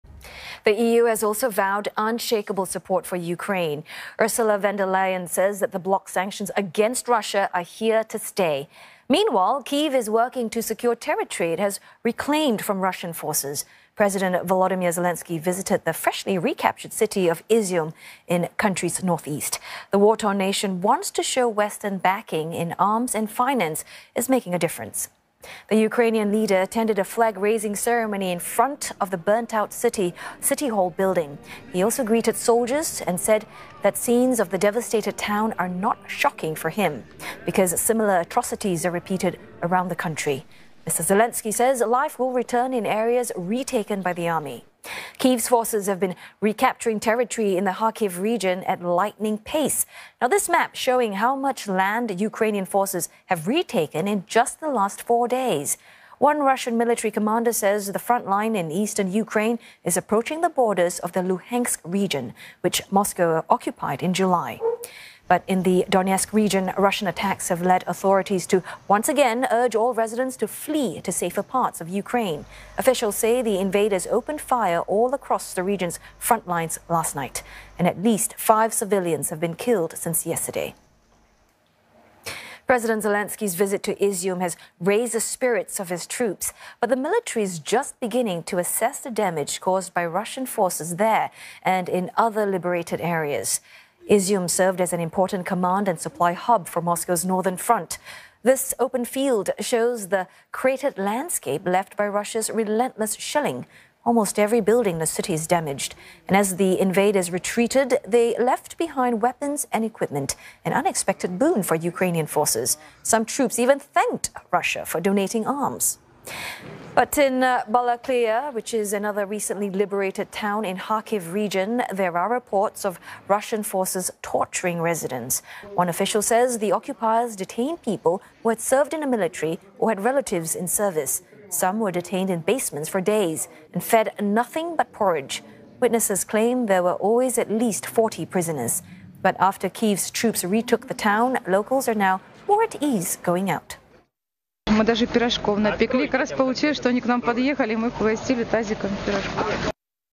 Ukraine's Zelenskyy visits retaken city of Izium, greets soldiers
The European Union has vowed unshakeable support for Ukraine. European Commission President Ursula von der Leyen has said the bloc's sanctions against Russia are here to stay. Meanwhile, Kyiv is working to secure territory it has reclaimed from Russian forces. President Volodymyr  Zelenskyy visited the freshly recaptured city of Izium in the country's northeast. The war-torn nation wants to show that Western backing in arms and finance is making a difference.